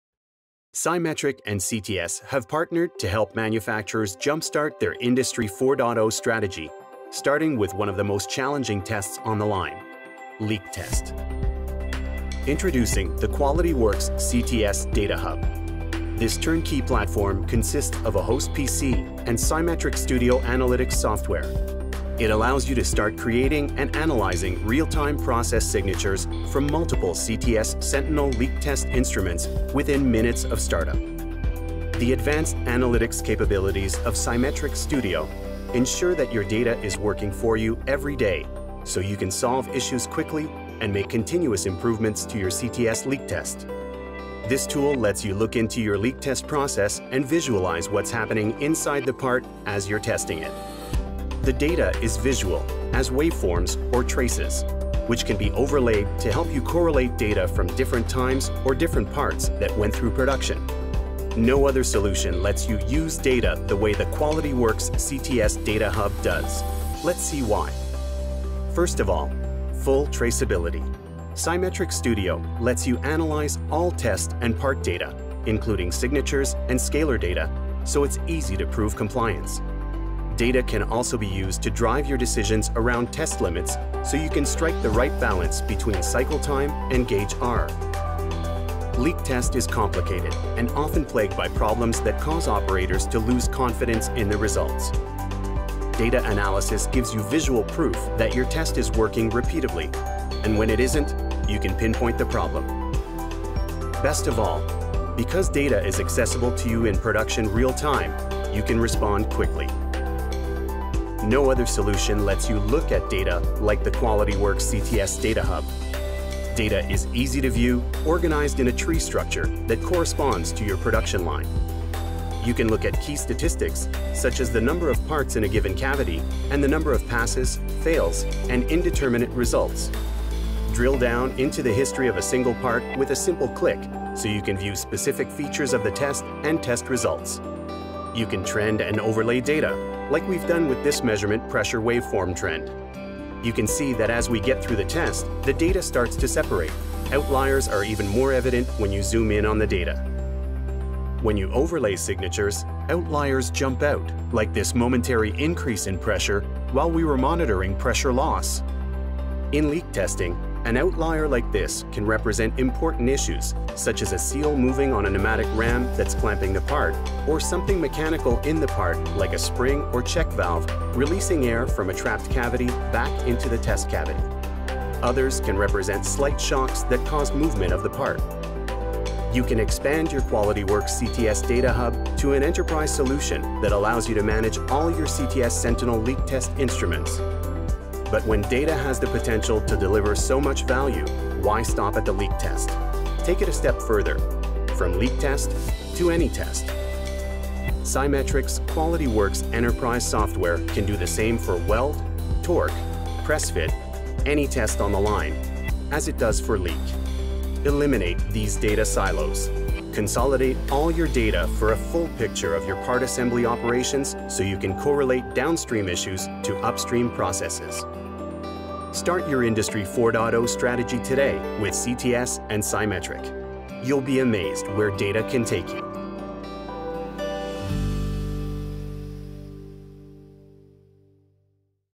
QualityWorX CTS DataHub
The QualityWorX CTS DataHub software connects to Sentinel leak test and sigPOD instruments at one or more stations along the production line to collect, store and analyze data.

This information is used for monitoring the testing process and to analyze historical data to identify trends and areas for improvement.